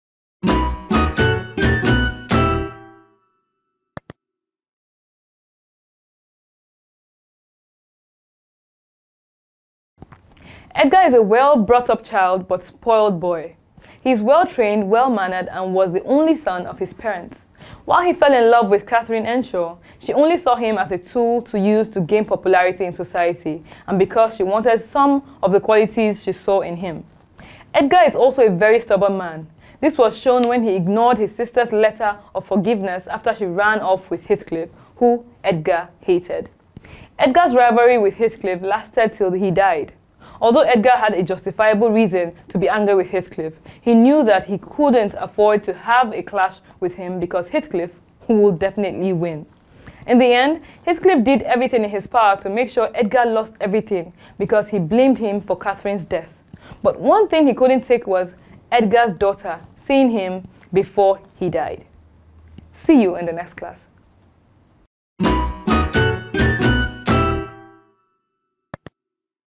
Wuthering Heights | Chapter Analysis Eglinton | Literature In English | SS2
While love seems to be the prevailing theme of Wuthering Heights, the novel is much more than a romantic love story. Intertwined with the (non-consummated) passion of Heathcliff and Cathy are hatred, revenge, and social class, the ever-prevailing issue in Victorian literature.